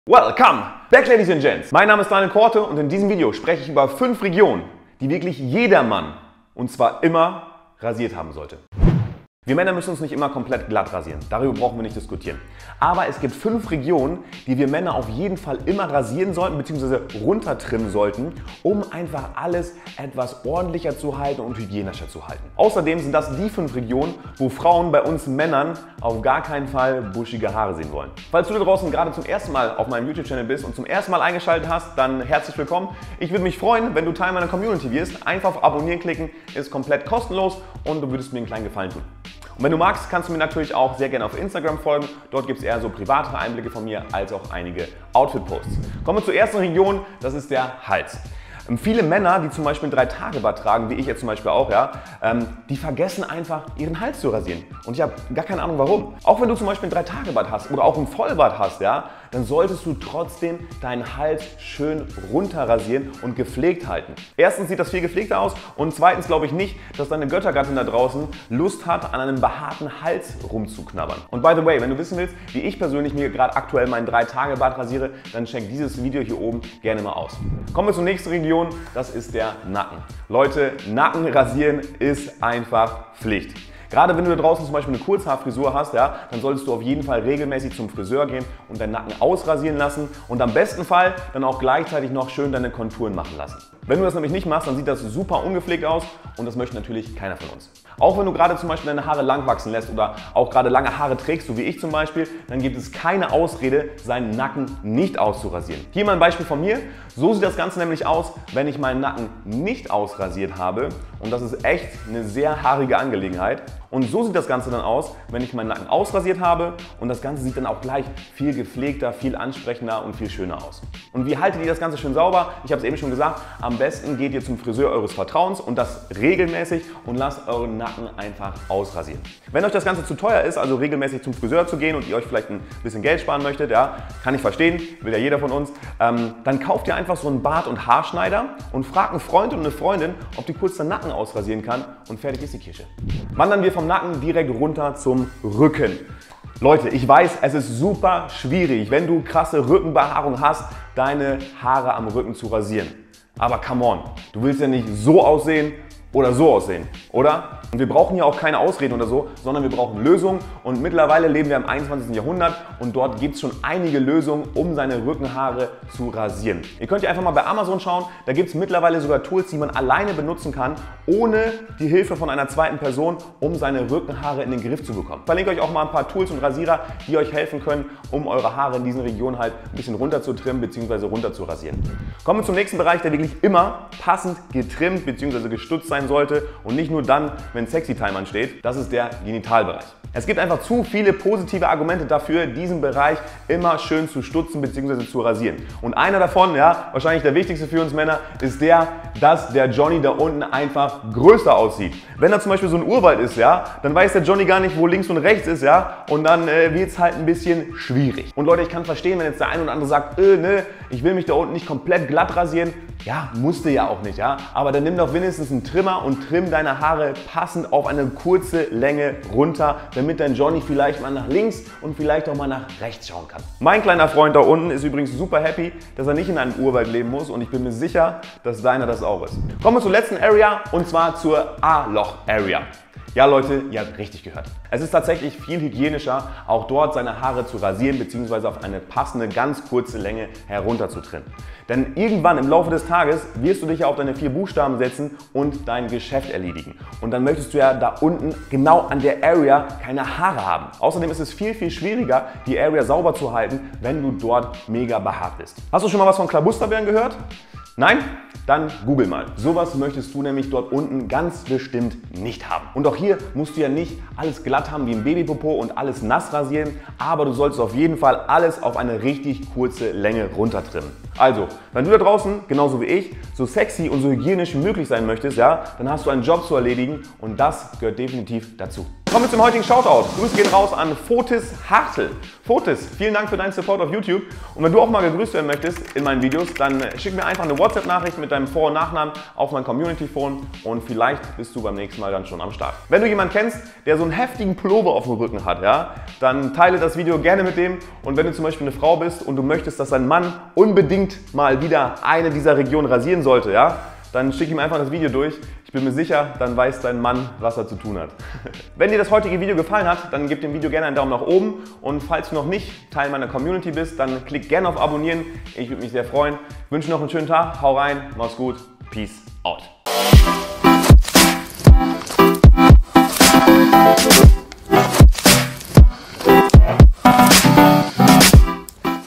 5 Regionen die JEDER MANN rasieren MUSS ● RASIEREN als MANN
Hey Leute,
heute spreche ich über 5 Regionen, die wirklich jeder Mann rasieren sollte und dazu zählt auch auch der Intimbereich lieber Männer!
Wenn du die unsicher bist, welche Regionen du als Mann rasieren solltest, weißt du nach diesem Video auf jeden Fall Bescheid. Egal ob Nacken rasieren, Hals rasieren, Rücken rasieren oder Intimbereich rasieren, auch das gehört zu unseren Aufgaben in der Männerpflege. Wenn ihr mehr Männerpflege Tipps haben wollt, schreibt es einfach in die Kommentare :)
Viel Spaß beim Rasieren und Trimmen!
Peace
DK